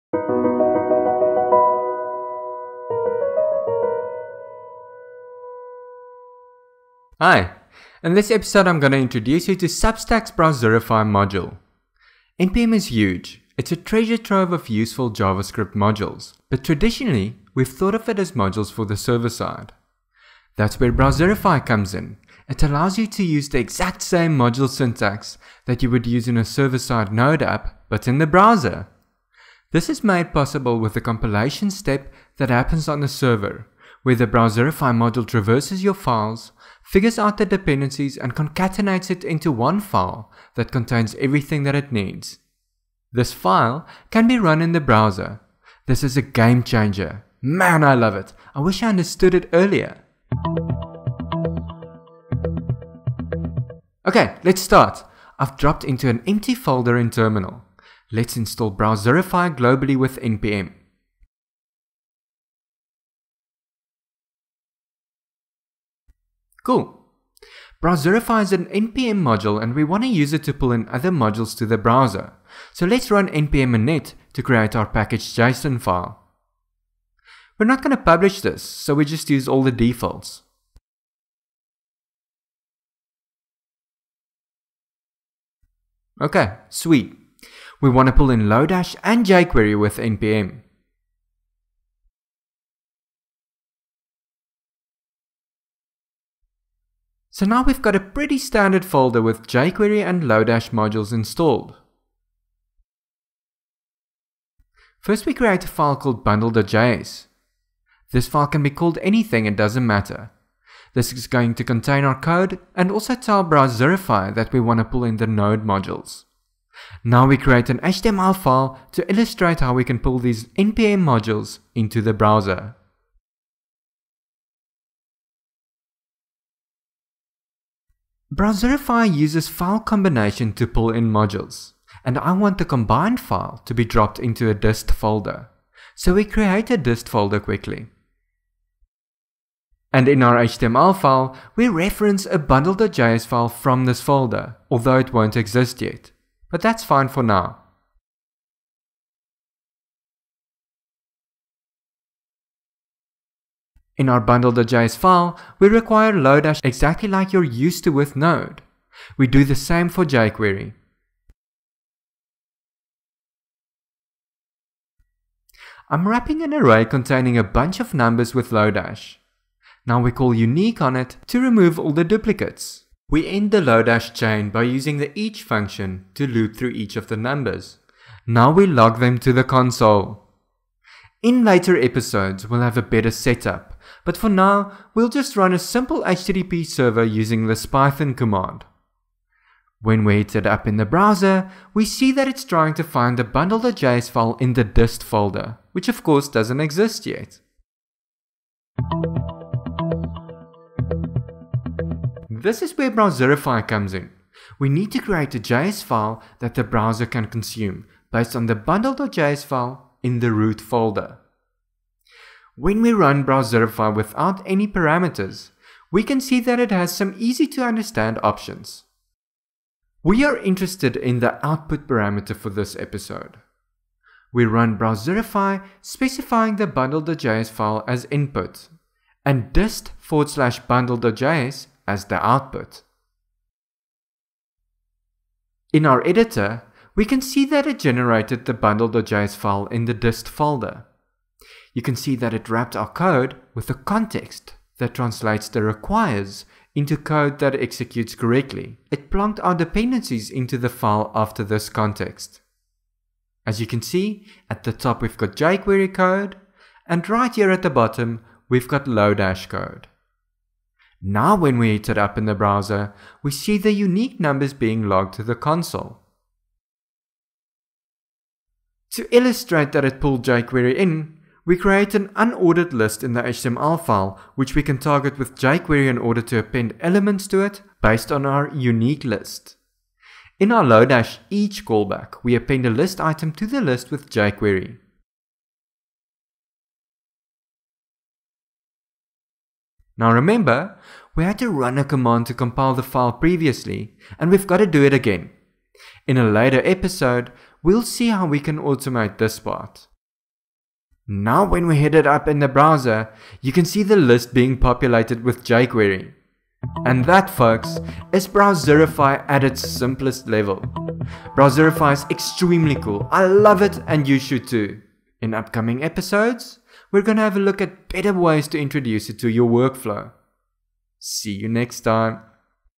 browserify: How to use npm modules in the browser
This video shows you how to use browserify in order to use npm modules for client side web applications.